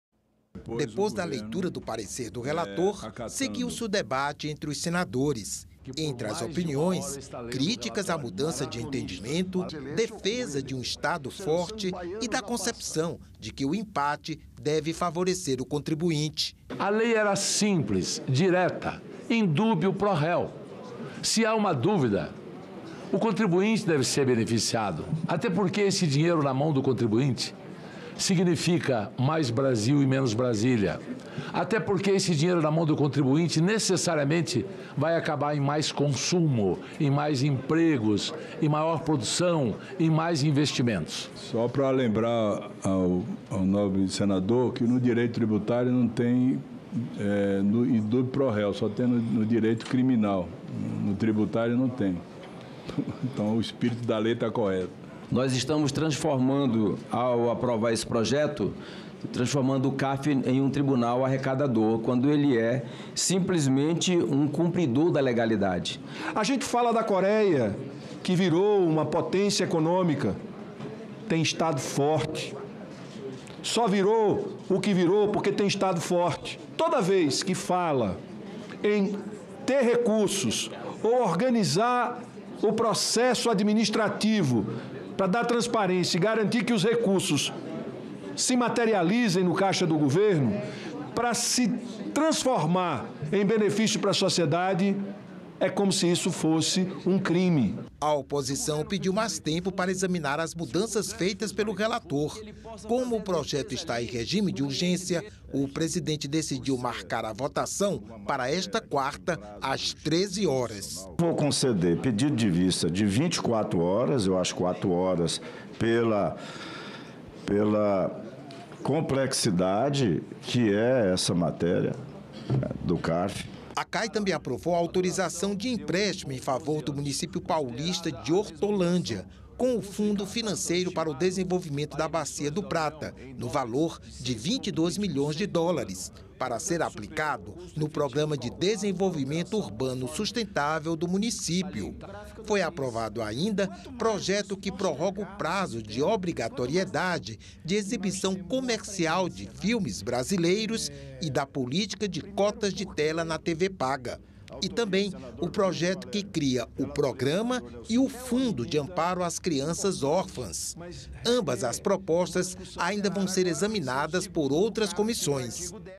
Proposta de mudança em julgamentos do Carf deve ser votada nesta quarta na CAE
Projeto que muda o procedimento do Conselho Administrativo de Recursos Fiscais (Carf) em caso de empate nos julgamentos foi discutido na Comissão de Assuntos Econômicos (CAE) nesta terça (22). O PL 2384/2023 estava na pauta da reunião, mas foi concedida vista coletiva e a proposta deve ser votada nesta quarta (23). O relator do texto é o senador Otto Alencar (PSD-BA). E a comissão aprovou autorização de empréstimo externo para Hortolândia (SP) e projeto que cria programa e fundo de amparo a crianças órfãs.